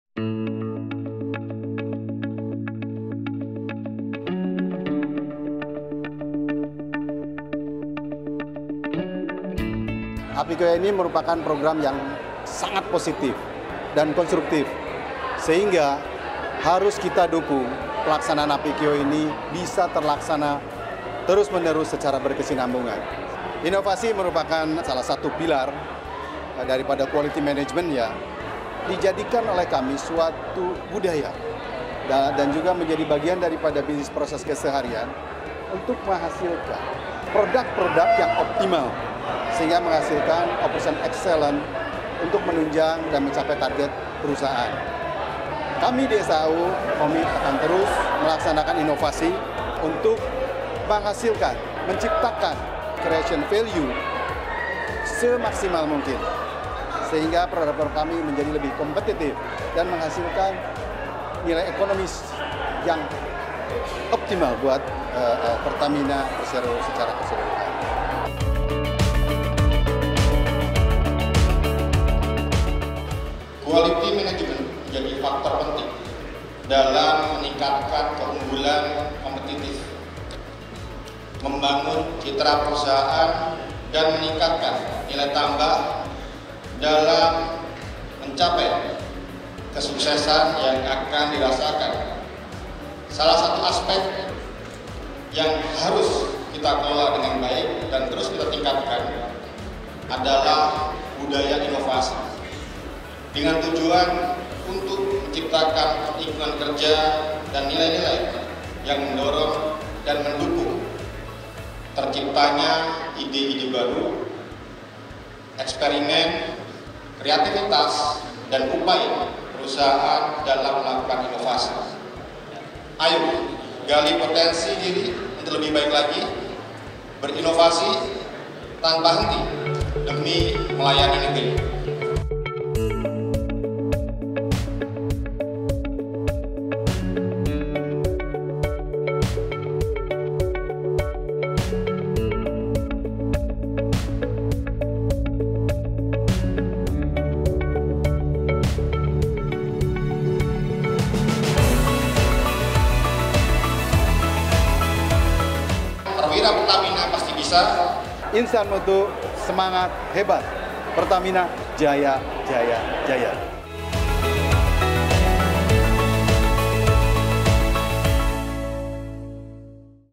APQ AWARD 2023, Pertamina Catat Penciptaan Inovasi Hingga Rp12 Triliun
Pertamina kembali menggelar Annual Pertamina Quality Award (APQA) 2023, suatu ajang internal perusahaan untuk menjaring  inovasi dari Perwira Pertamina Group. Sebanyak 154 risalah unggulan program APQA tahun ini teruji mampu menciptakan nilai (value creation) hingga Rp12,02 triliun.

“Kami berharap semangat inovasi yang sudah terbangun ini dapat terus dilanjutkan dan terus dipertahankan, serta gagasan dan produk inovasi yang sudah tercipta agar dilaksanakan secara berkelanjutan,” ungkap Direktur Sumber Daya Manusia Pertamina, M. Erry Sugiharto.